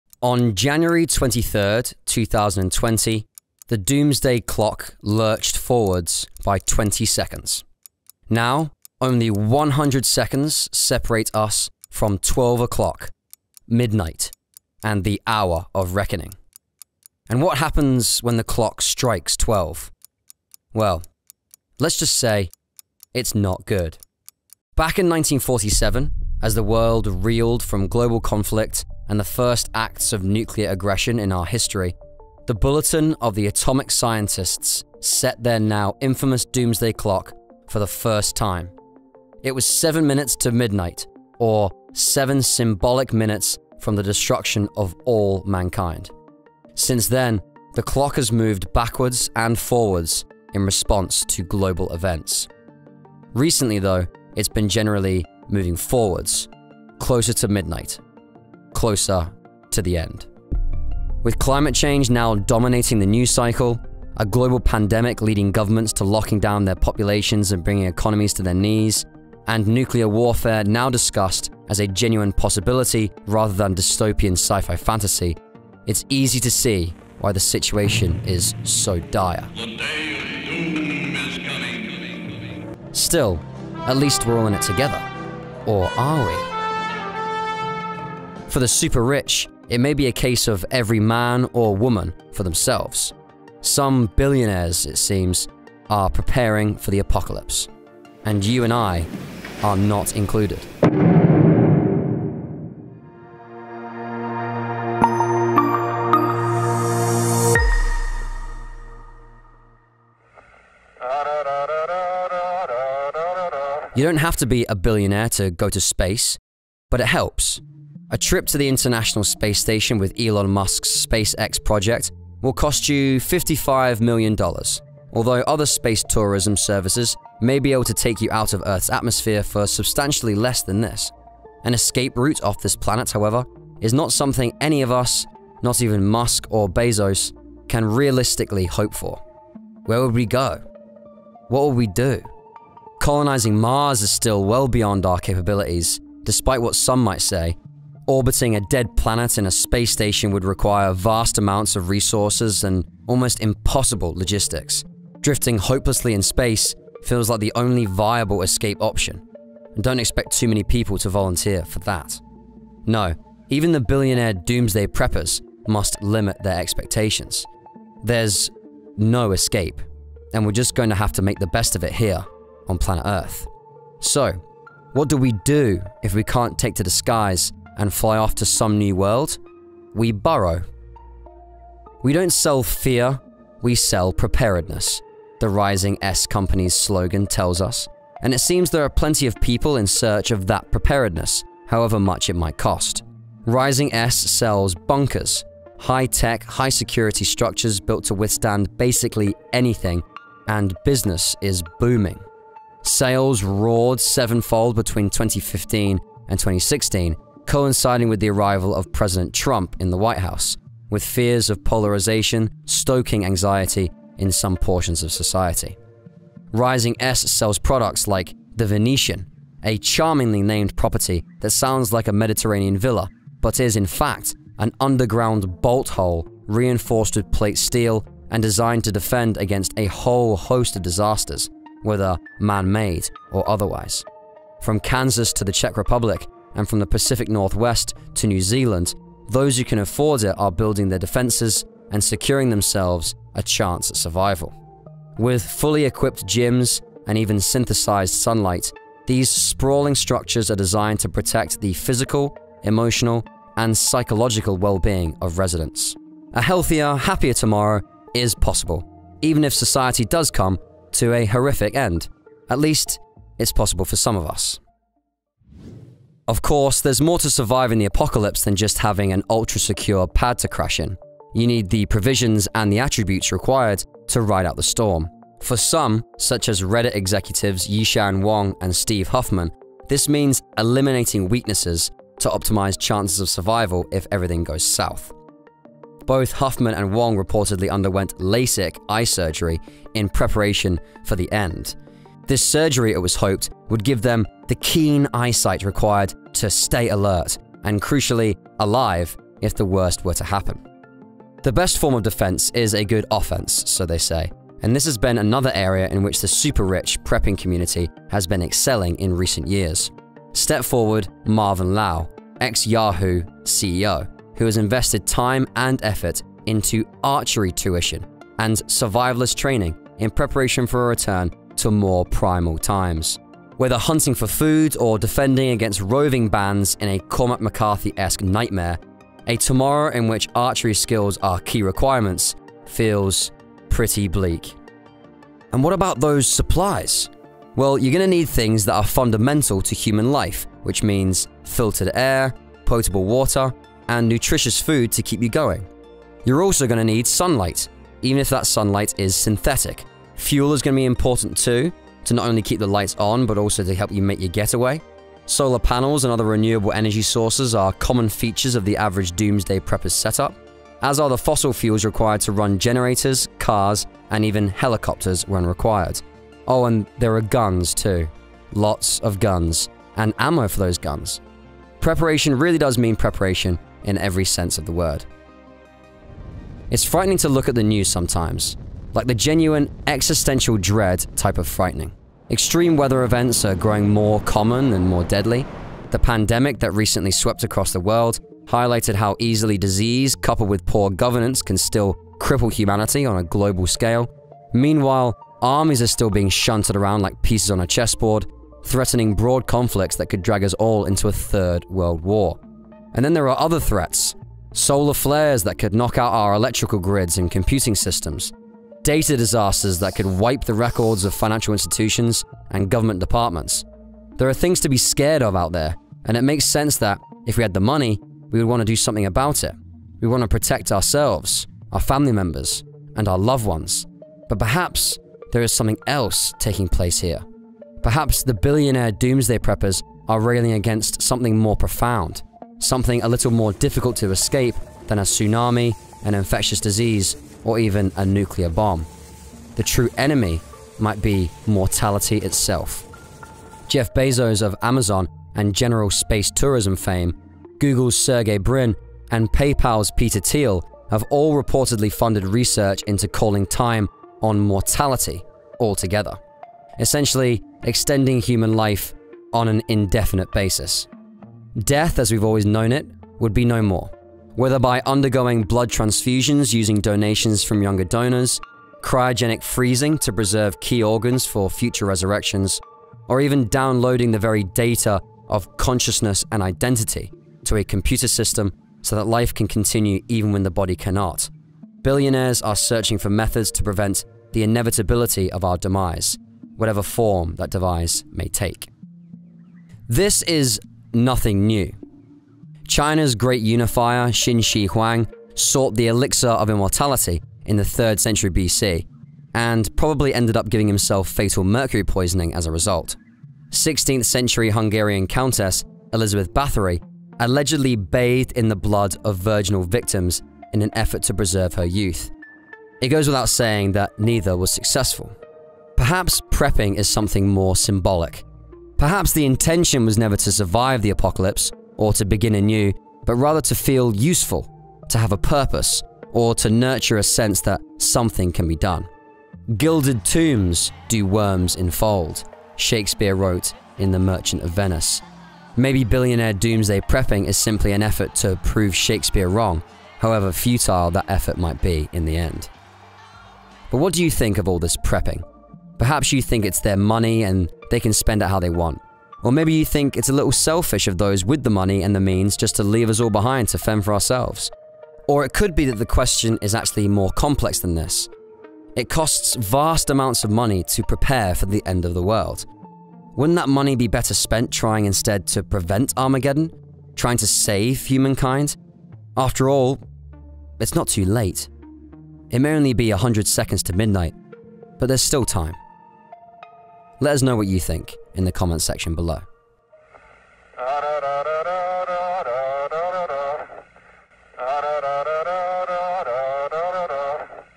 The ALARMING Rise of Billionaire Doomsday Preppers: What are they Doing, Where are they Going & Why?
Some billionaires, it seems, are preparing for the apocalypse.

🎬Video Credits:
Narrator - Pat
Editors - Kshitiz, Shantanu koli
Writer - John B
Quality Control - Dan

Chapters:

0:00 Introduction
1:49 Fight or Flight — the Only Option Might Be to Ride It Out
4:25 Failing to Prepare Is Preparing to Fail
6:15 The Existential Dread at the Heart of It All
9:02 Conclusion